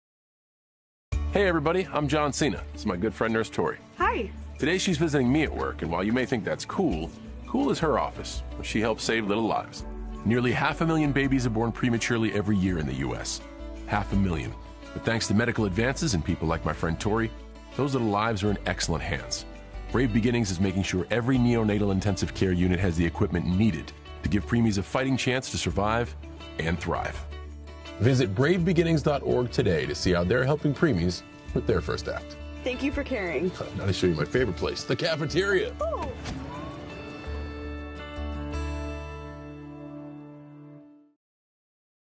John Cena Stars in NEW Brave Beginnings PSA
John Cena has officially partnered with Brave Beginnings to have this PSA show in theaters this Summer.
Brave Beginnings is a non-profit organization that supports NICU's in need of equipment to save NICU babies. They provide ventilators, stethoscopes, neopuffs, and much more!
Brave Beginnings has donated over $7.5million to around 200 different hospitals across the U.S. Brave Beginnings has saved many and continues to save many premature born babies all across the United States.